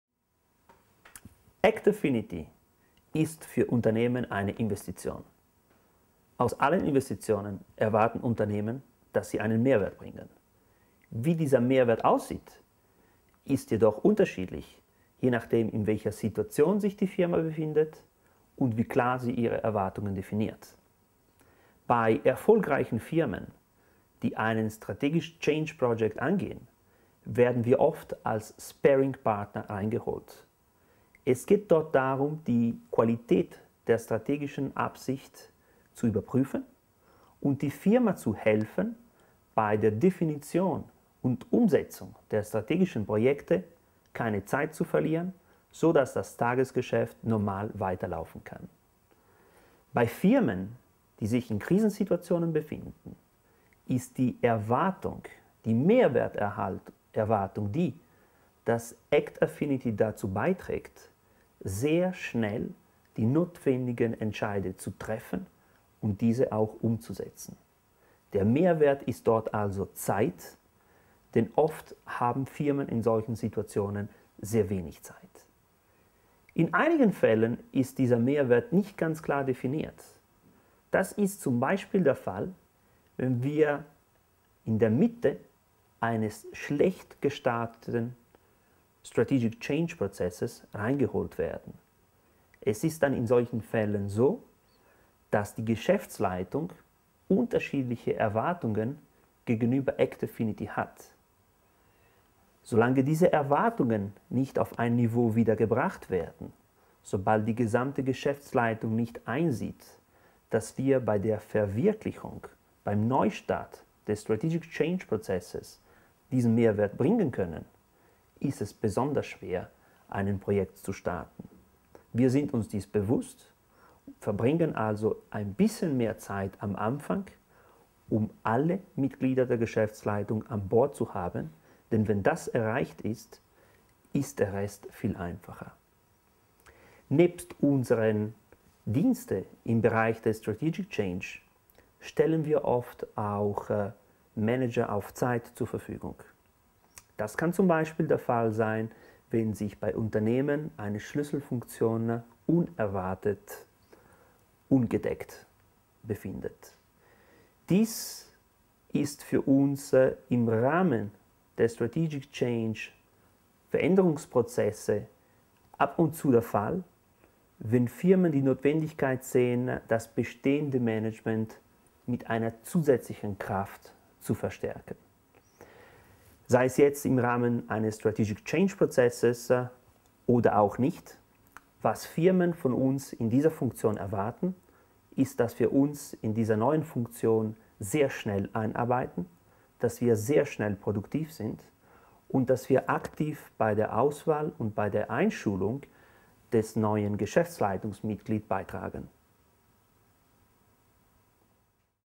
Dr. Max Tschabuschnig -- Was Kunden von Act Affinity AG erwarten
Act Affinity AG Partner Dr. Max Tschabuschnig zu der Frage, was Unternehmen an Unterstützung in ihren  Veränderungsprozessen von Act Affinity AG erwarten.